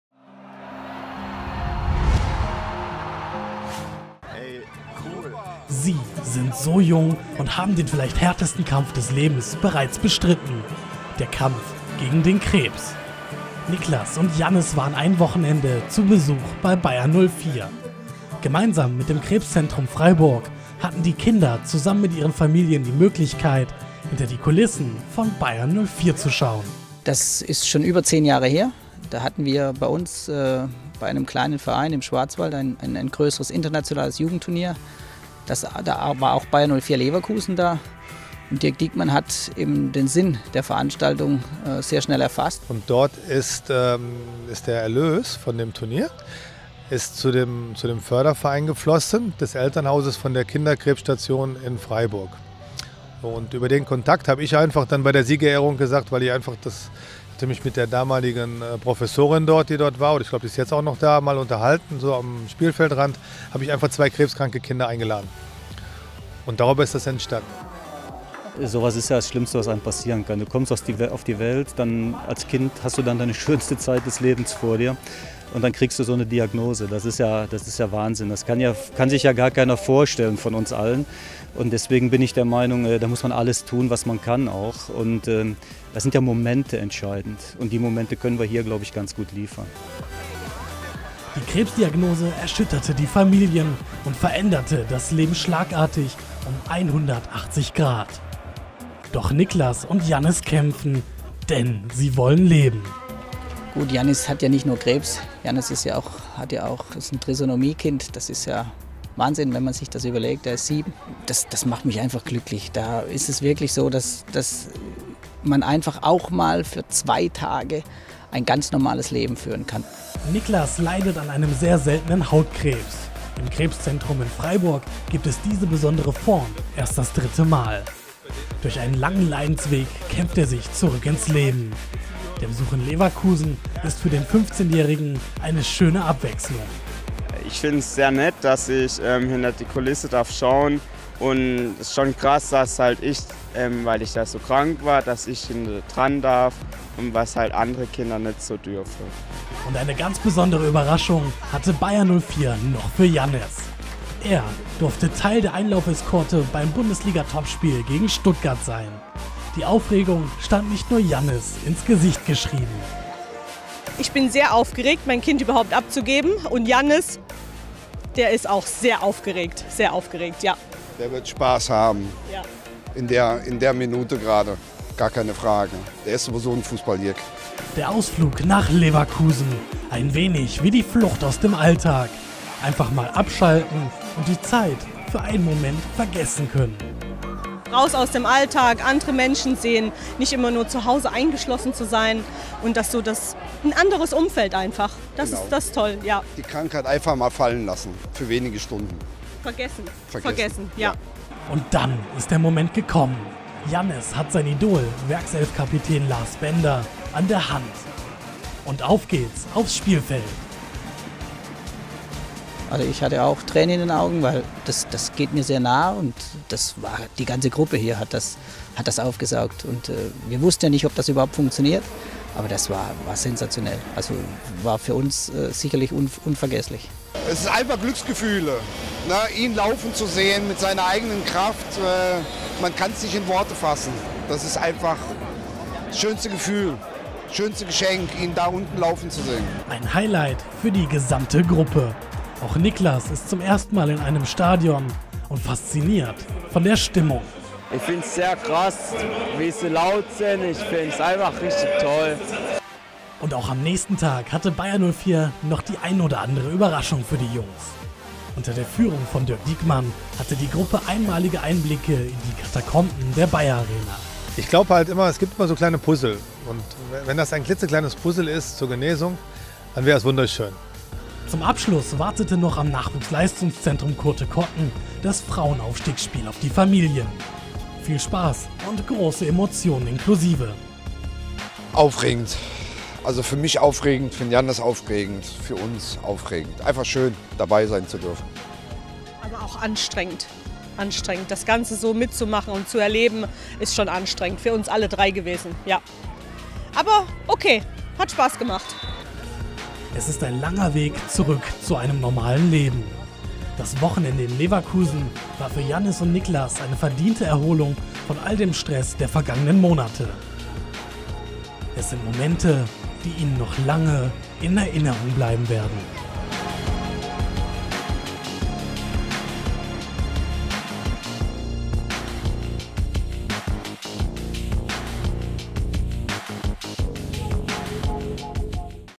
Krebskranke Kinder bei Bayer 04 Leverkusen
Der Förderverein für krebskranke Kinder e.V. Freiburg ermöglichte im Mai 2018 zwei krebskranken Kindern und ihren Familien den Besuch bei Bundesligist Bayer 04 Leverkusen. Die Kinder erlebten ein aufregendes Wochenende voller Fußball-Abenteuer. Bayer 04-TV hat die Familien begleitet und die Erlebnisse dokumentiert.